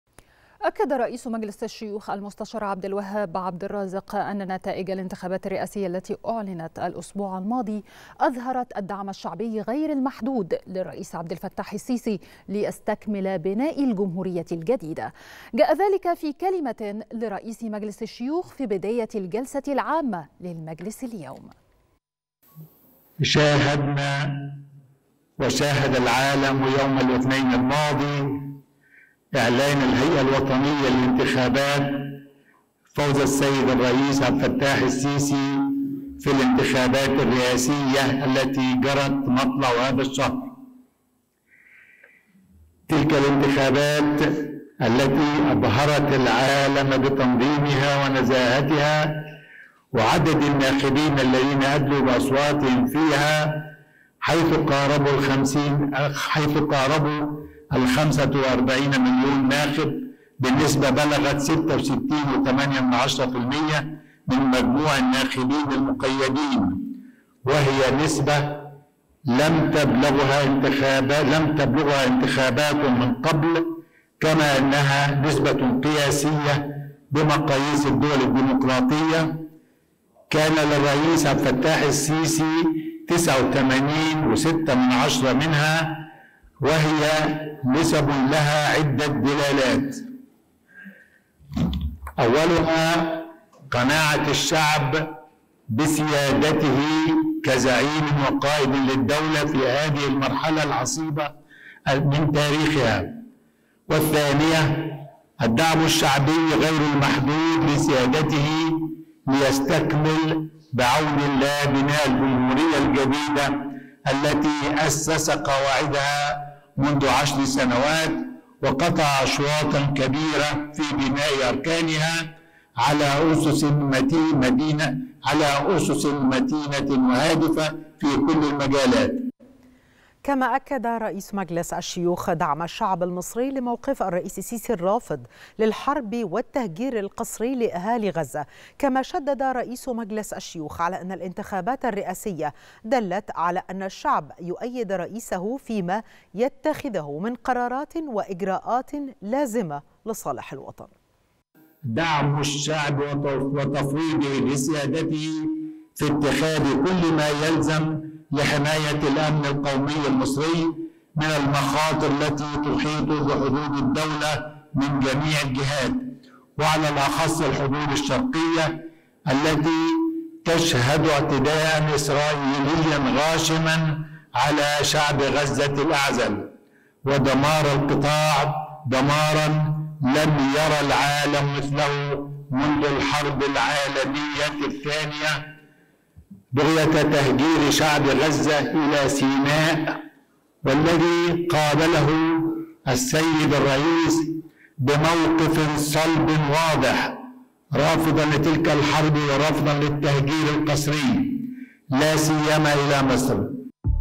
رئيس مجلس الشيوخ: نتائج الانتخابات الرئاسية أظهرت الدعم الشعبي غير المحدود للرئيس السيسي
نشرة الثالثة | رئيس مجلس الشيوخ: نتائج الانتخابات الرئاسية أظهرت الدعم الشعبي غير المحدود للرئيس السيسي